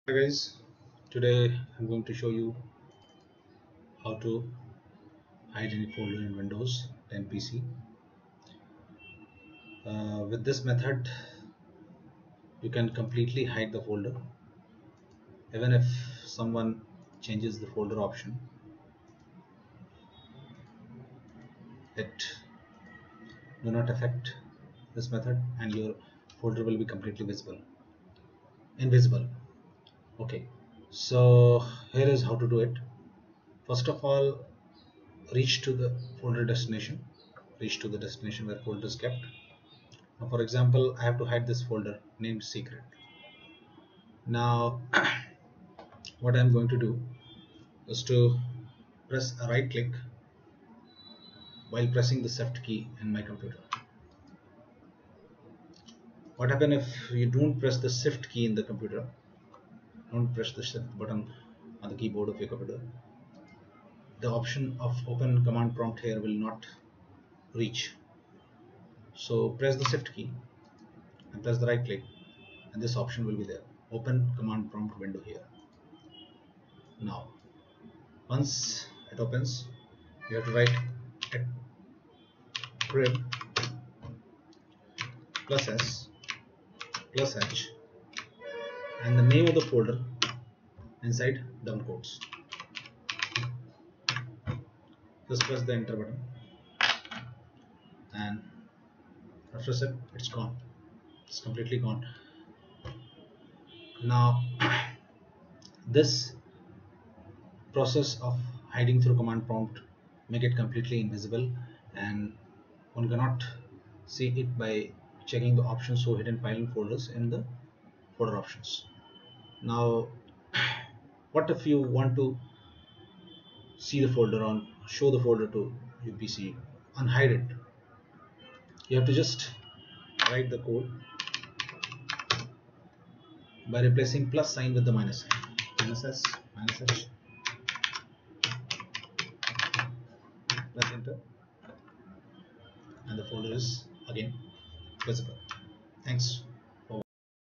How to hide a Folder completely by CMD in windows 10
How to hide a Folder completely by Command prompt CMD in windows 10/8/7 PC. One can not unhide it by even checking the show hidden file and folder option also.